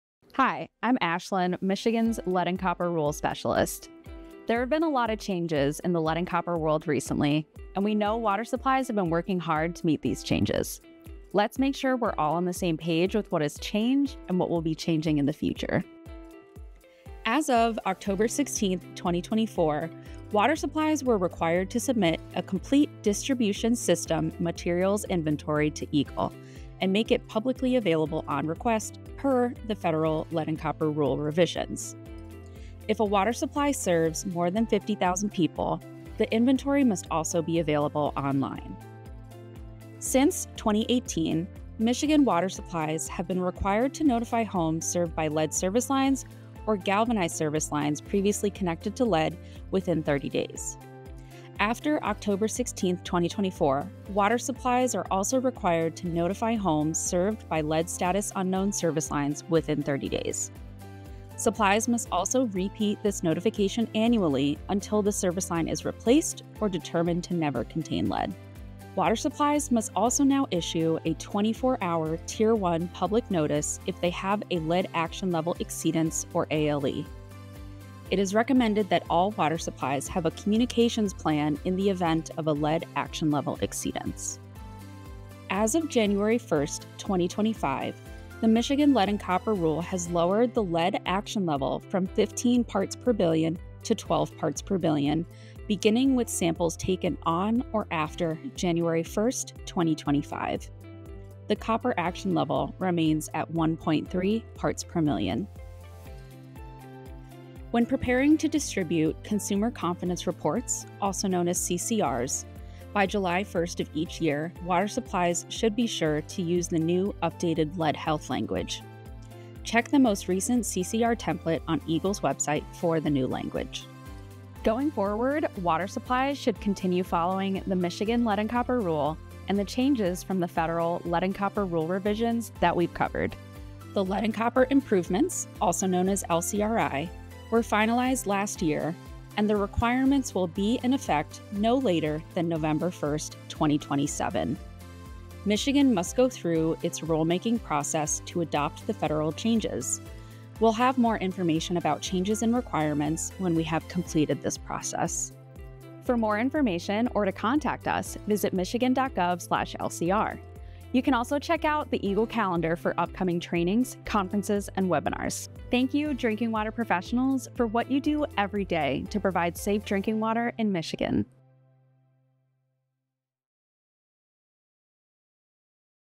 Lead and Copper Rule Changes
There have been a lot of changes in the lead and copper world recently, and we know water supplies have been working hard to meet these changes. Let’s make sure we are all on the same page with what has changed, and what will be changing in the future.